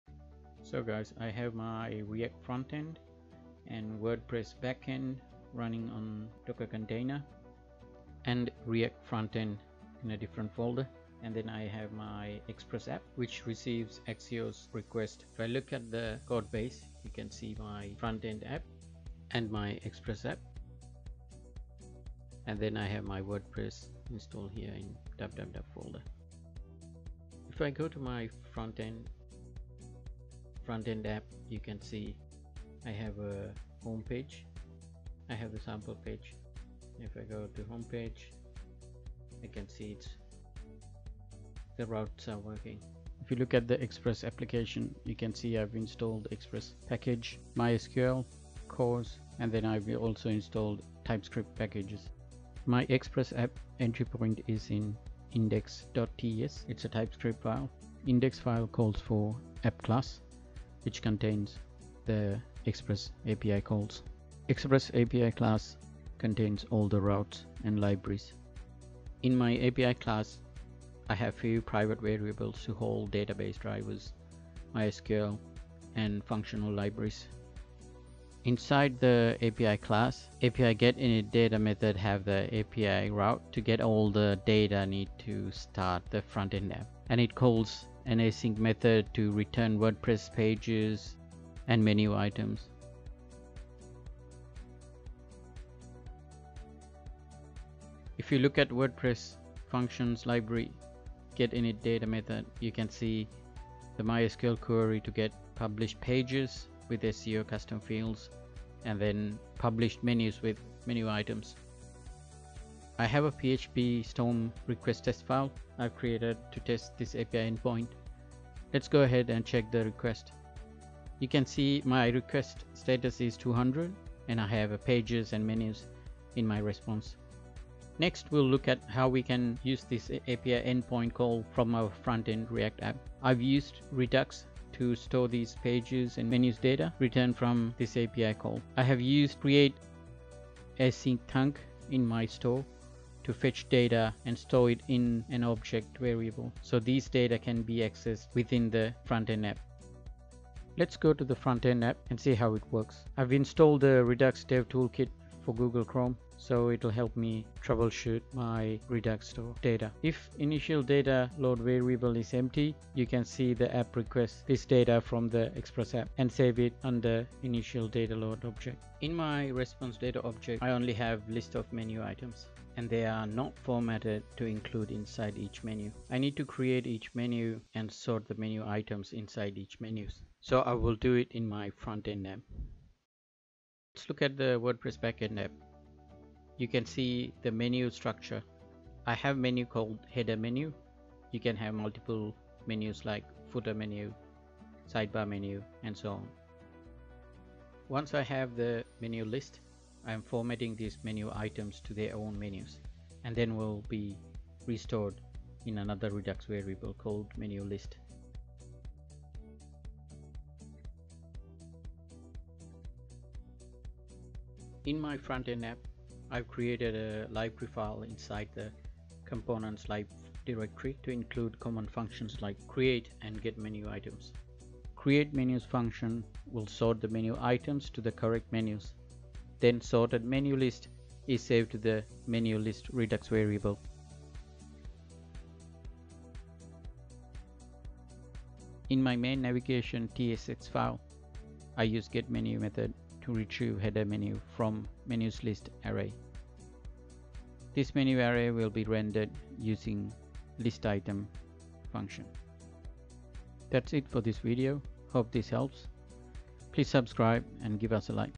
WordPress backend, React frontend with supporting Express app
This video demonstrates how to use WordPress as the backend and Rect as front front-end website.

It also uses the express supporting app to retrieve data from the Wordpress database.

The video explains How to get WordPress Pages and Menus in your React app.

You can find the code base for front end react app